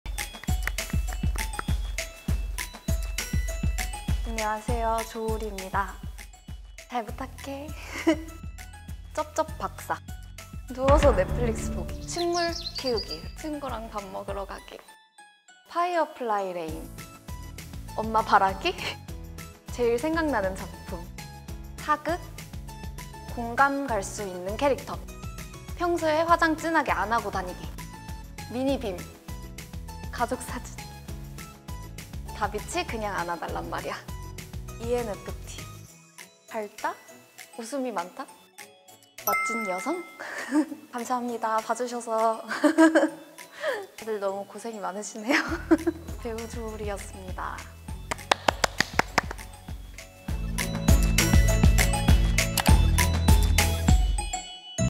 보기만해도 눈이 번쩍✨ 뜨이는 스윗한 조우리와의 1분인터뷰 조우리
1min_interview with sweet JoWooRi making our eyes open✨｜Jo Woo Ri